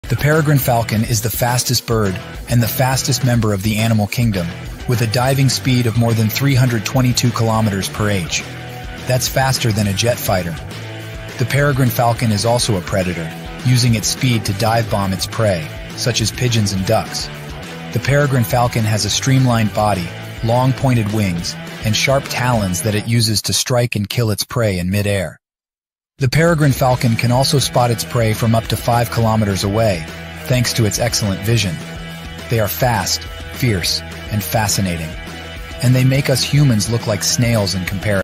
Peregrine Falcons: The Fiercest and Fastest Predators of the Sky 👀
If you think you're fast, think again. There are some animals out there that can outrun you in a blink of an eye. Here are the 3 fastest animals in the world-
The cheetah is the fastest land animal, capable of reaching speeds of up to 70 mph (112 km/h). That's faster than most cars on the highway! The cheetah can accelerate from 0 to 60 mph (97 km/h) in less than three seconds, which is faster than some sports cars. The cheetah is also a predator, using its speed to catch its prey, such as gazelles and impalas. However, the cheetah can only maintain its top speed for short distances, so it has to be quick and smart to catch its dinner.

The sailfish is the fastest fish in the ocean, with an estimated top speed of at least 67 mph (110 km/h). That's faster than a bullet train! The sailfish is also a predator, using its speed to hunt down other fish, such as sardines and anchovies. The sailfish has a long, sharp bill that it uses to slash and stun its prey, and a large dorsal fin that it can raise or lower to help it maneuver in the water. The sailfish can also change its color to blend in with its surroundings or communicate with other sailfish.

The peregrine falcon is the fastest bird and the fastest member of the animal kingdom, with a diving speed of more than 185 mph (322 km/h). That's faster than a jet fighter! The peregrine falcon is also a predator, using its speed to dive-bomb its prey, such as pigeons and ducks. The peregrine falcon has a streamlined body, long pointed wings, and sharp talons that it uses to strike and kill its prey in mid-air. The peregrine falcon can also spot its prey from up to 3 miles (5 km) away, thanks to its excellent vision.

These are just some of the amazing animals that can achieve incredible speeds in nature. They are fast, fierce, and fascinating. And they make us humans look like snails in comparison.